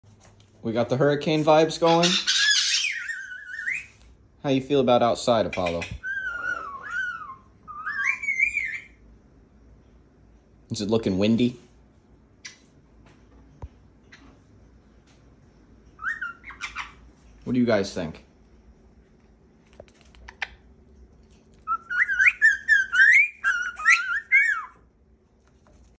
The Comfy Before the Storm
Vibe checking the birds while Hurricane Ian starts to come in.